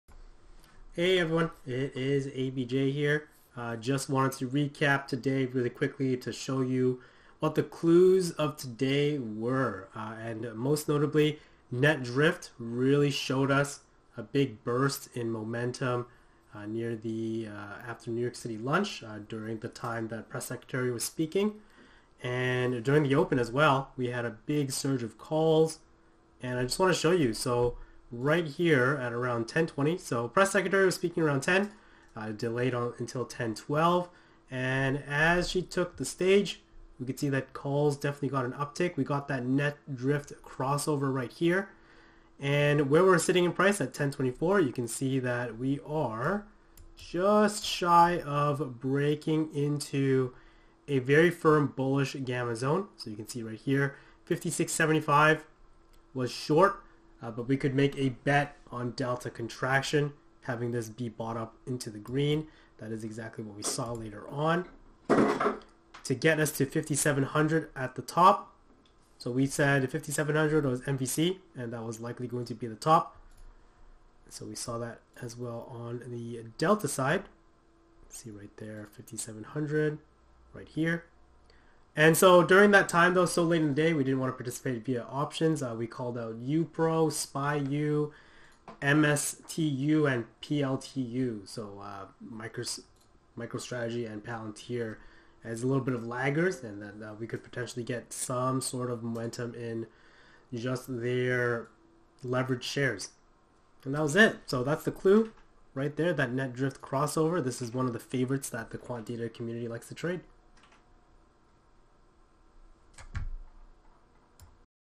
Net Drift Crossover - A QuantData FAVOURITE Trade!
Showcasing how today's Net Drift Crossover was a CLUE that we would rally into the close.

Join our free community!